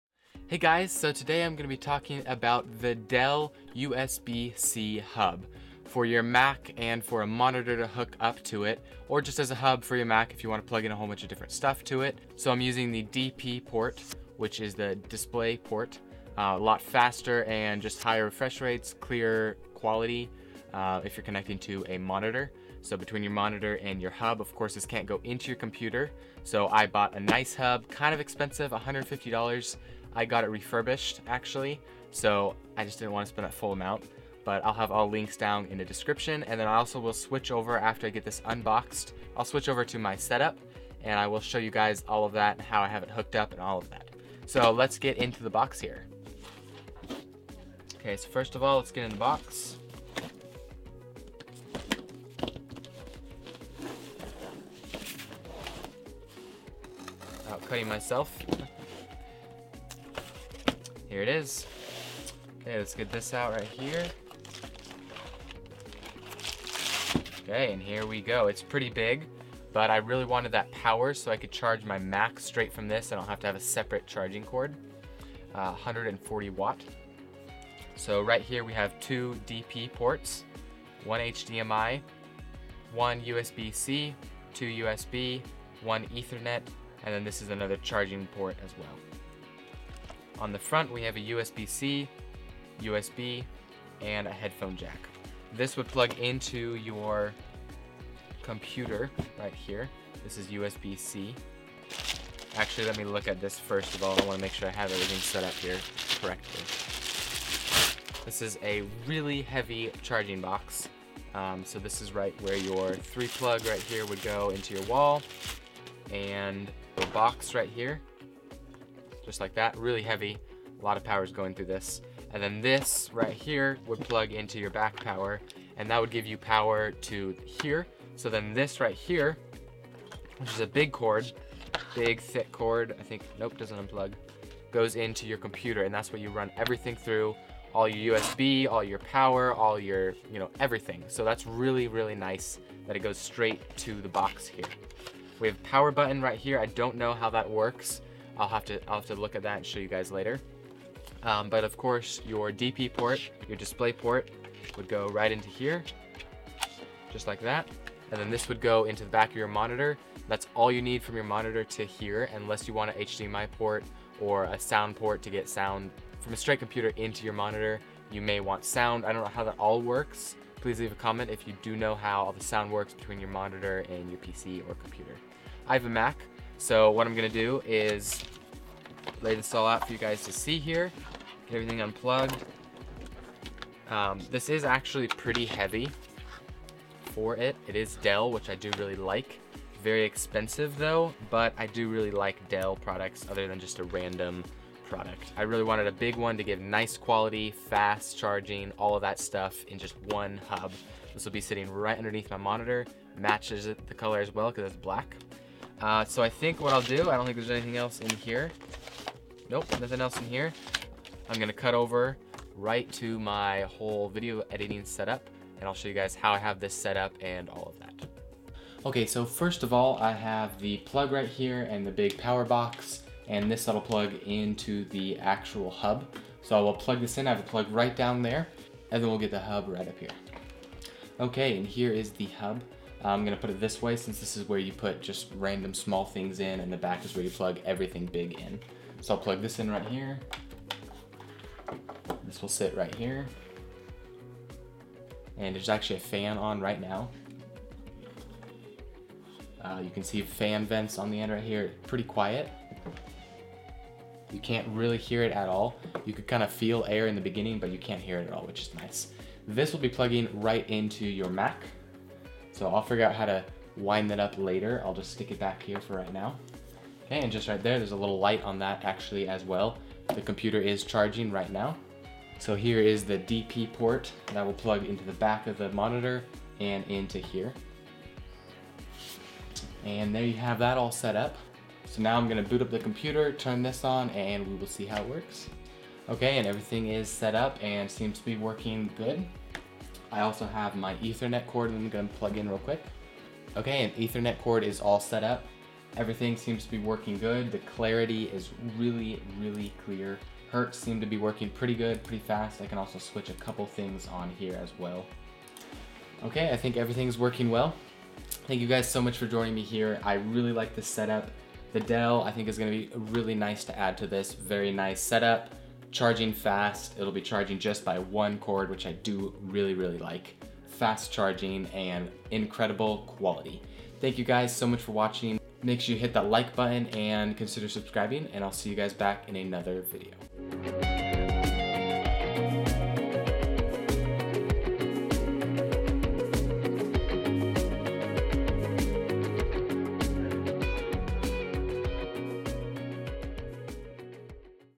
Powered USB-C Docking Station for Your MacBook Pro - Dell Hub
I just got my Dell USBC Hub for my MacBook Pro to my monitor and I absolutely love it.

Dell Hub on Amazon